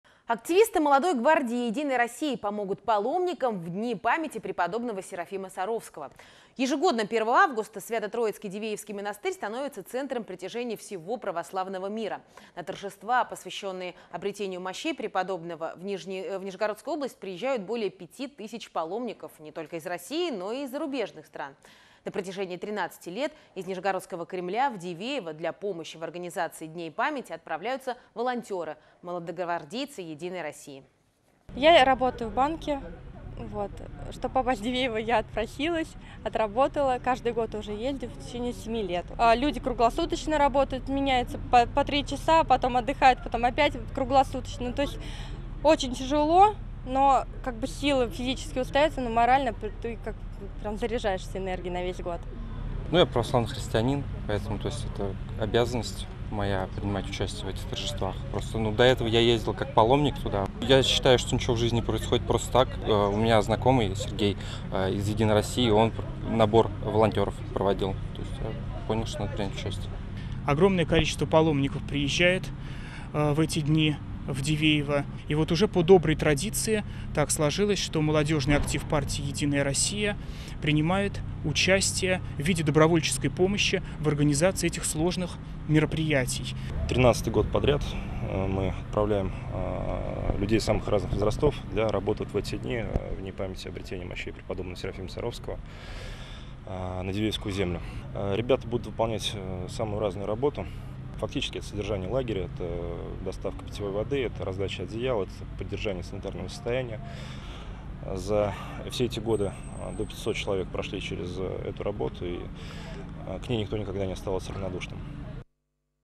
Активисты Молодой гвардии «Единой России» помогут паломникам
В Дни памяти преподобного Серафима Саровского. Ежегодно 1 августа Свято-Троицкий Дивеевский монастырь становится центром притяжения всего православного мира. На торжества, посвященные обретению мощей преподобного Серафима Саровского, в Нижегородскую область приезжает более пяти тысяч паломников не только из России, но и из зарубежных стран. На протяжении 13 лет из Нижегородского кремля в Дивеево для помощи в организации Дней памяти отправляются волонтеры – молодогвардейцы «Единой России».